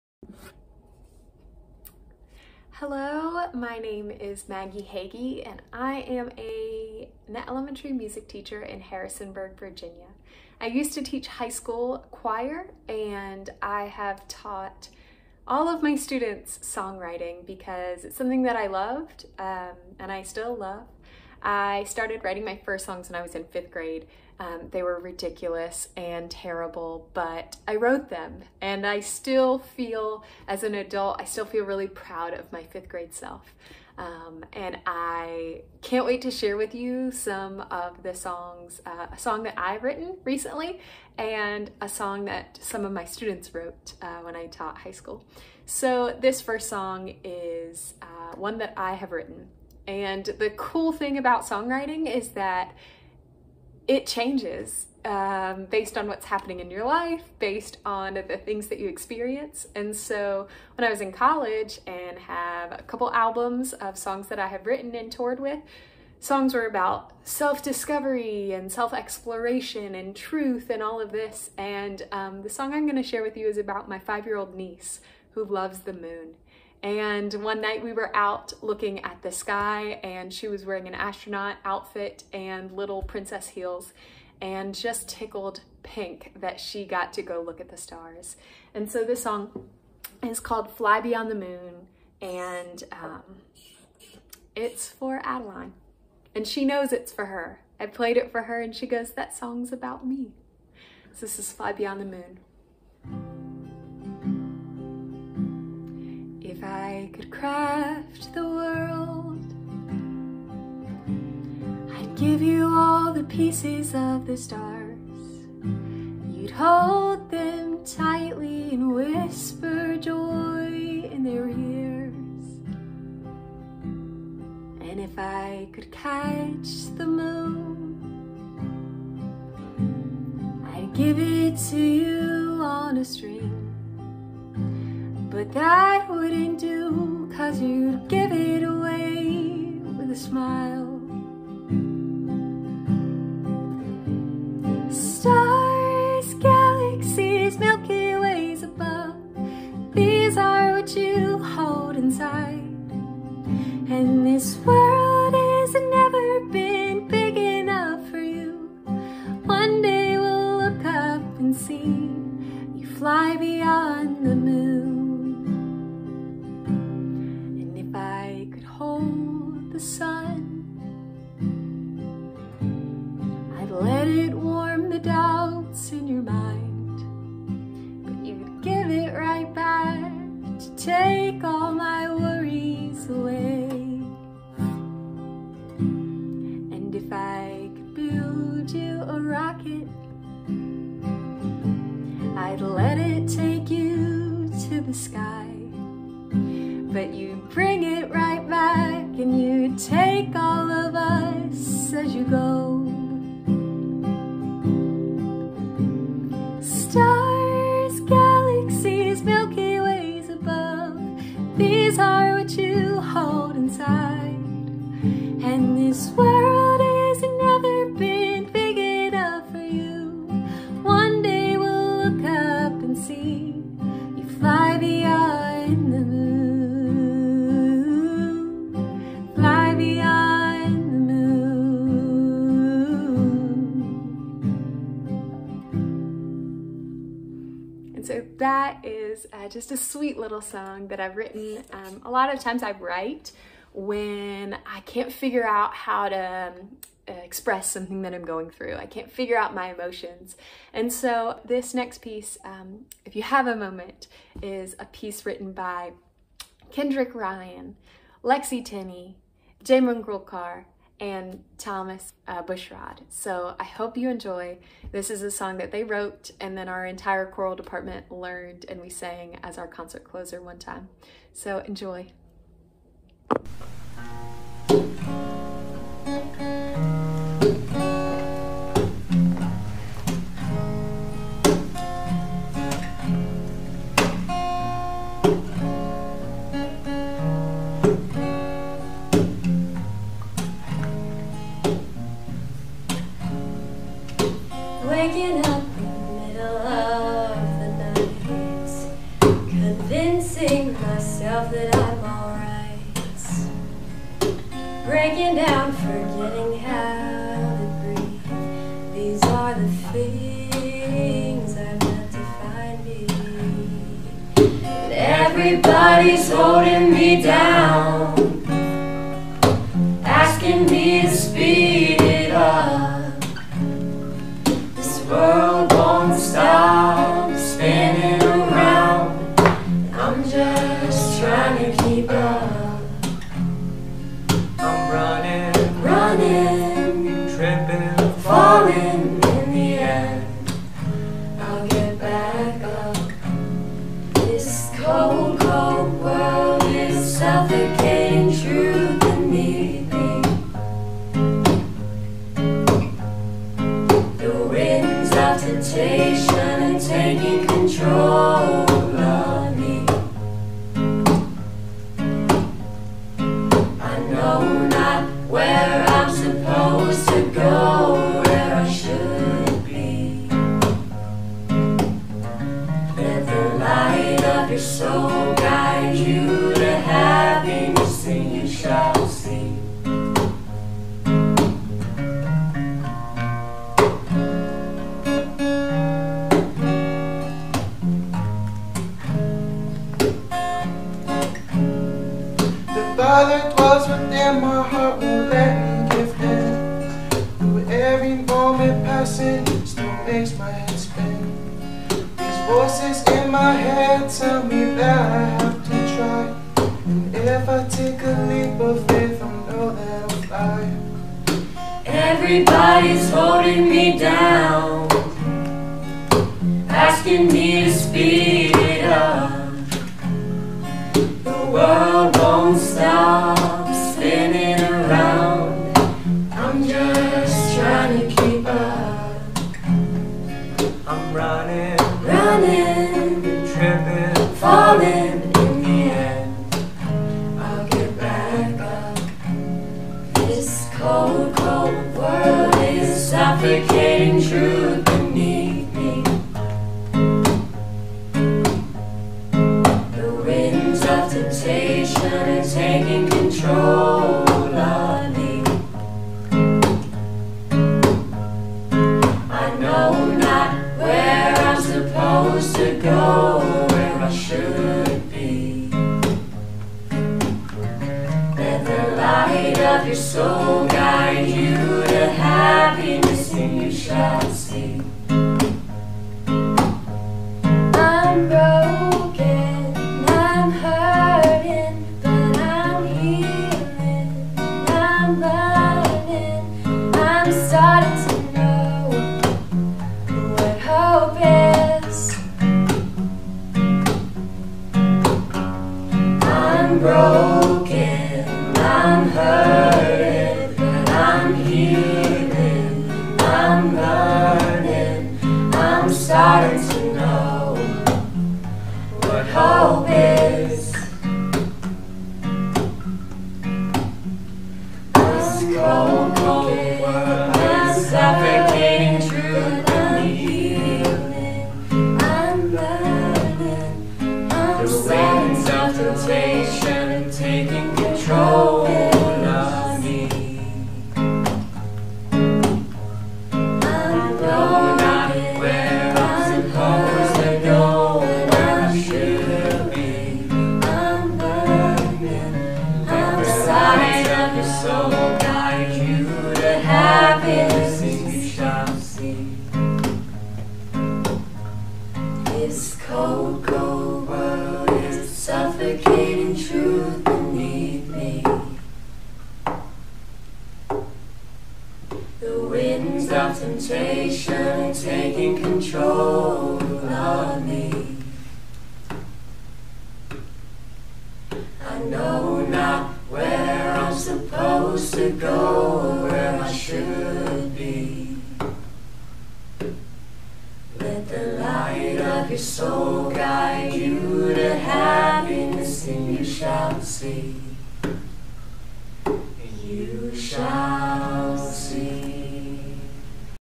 Maggie's Song: Two Examples, All ages!
In this video, Music Teacher Maggie shows us a song she wrote, inspired by her niece, as well as a song her own high school Choir students wrote that went on to be performed by the whole choir department. Enjoy and be inspired! :)